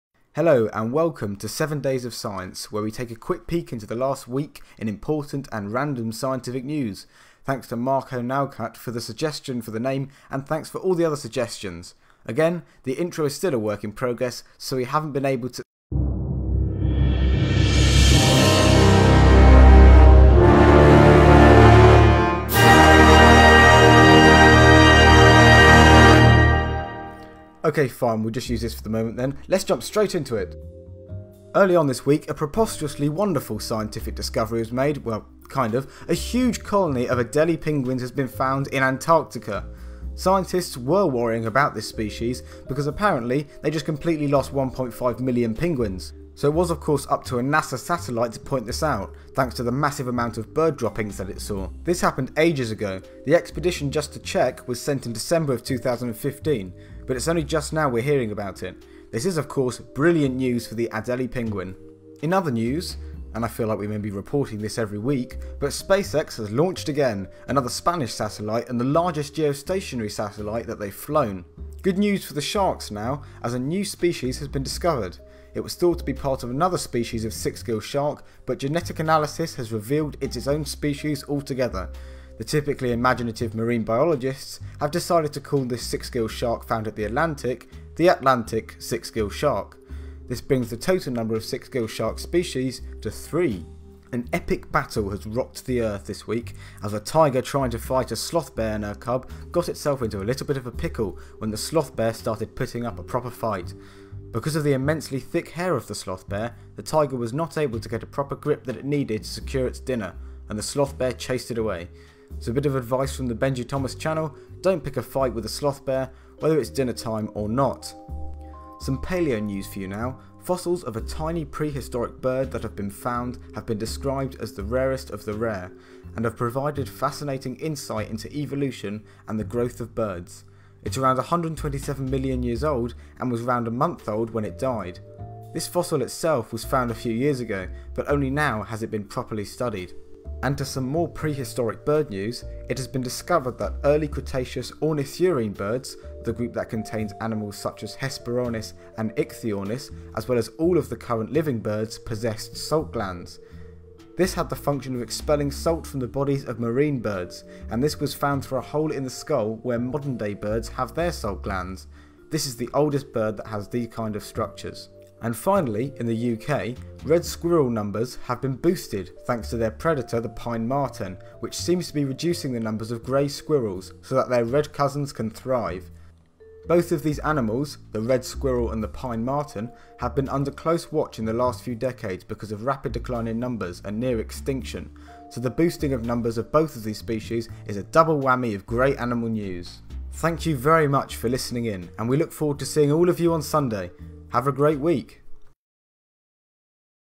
Tiger vs Sloth Bear & 1.5 Million More Penguins - 7 Days of Science
This week there's been all sorts of news, from the discovery of a penguin super-colony to a tiny, fossilised baby bird from Spain. SpaceX launched some new satellites as well, and a Tiger tried to take on a Sloth Bear in India.